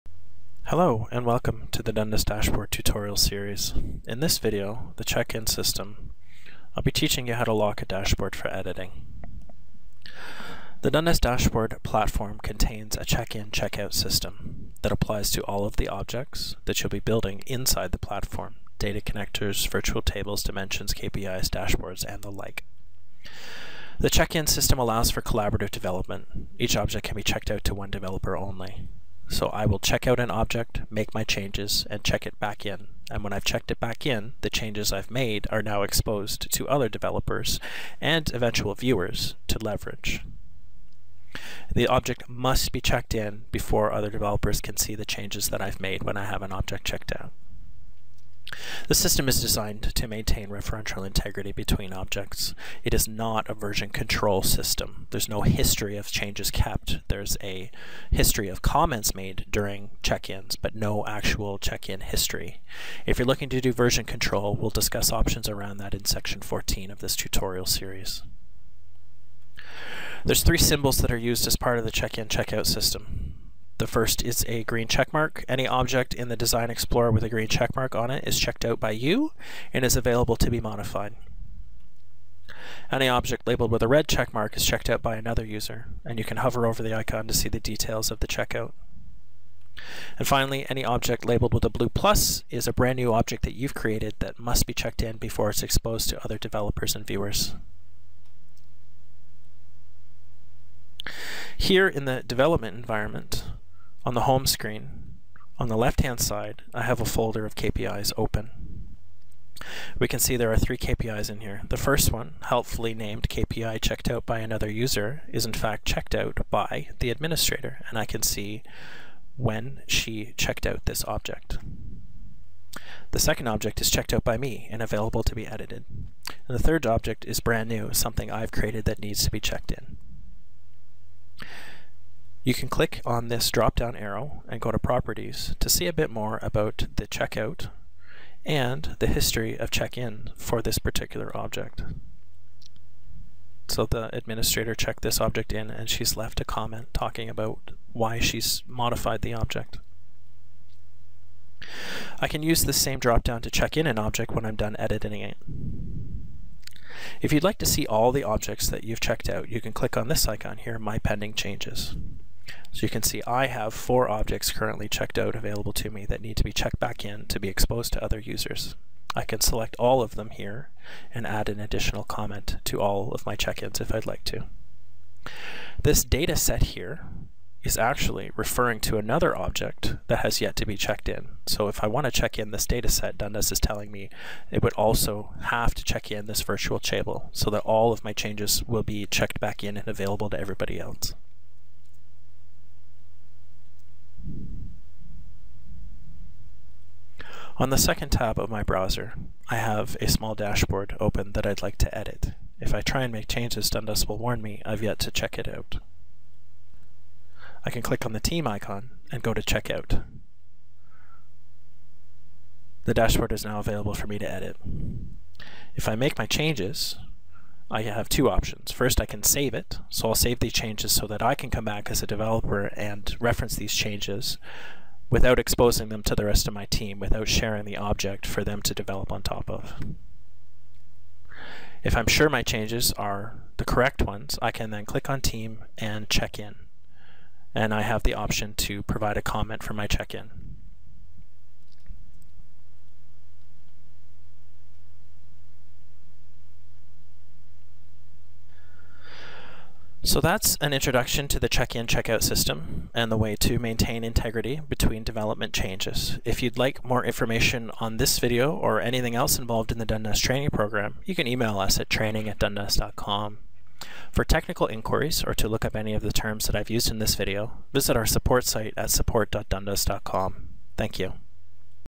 1.06 Fundamentals - Check In System
In this video, learn about the check-in/check-system used within Dundas Dashboard to maintain referential integrity inside a multiple developer environment.